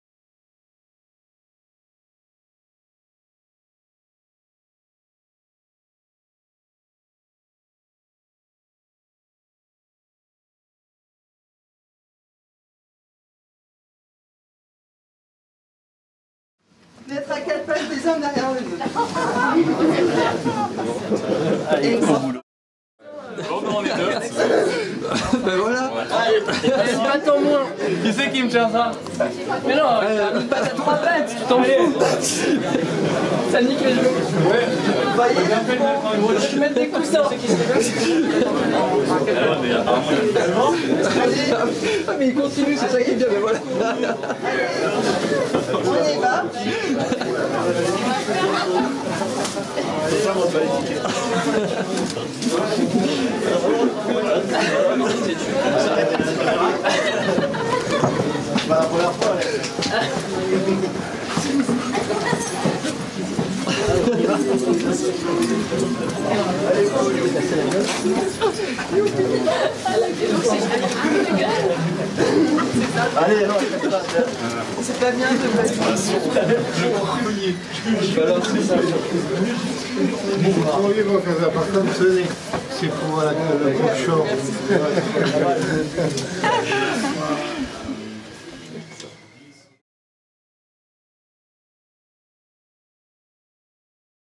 Festival 2011: Human Centipede live
Aperçu de la 2e Fantastique Semaine du Cinéma
qui s'est déroulée du 24 au 30 octobre 2011 à Nice
Théâtre de la Cité & Cinéma Mercury

Jour 2
introduction du film THE HUMAN CENTIPEDE: FIRST SEQUENCE de Tom Six
Les spectateurs sont rentrés à quatre pattes et en file indienne dans la salle de cinéma !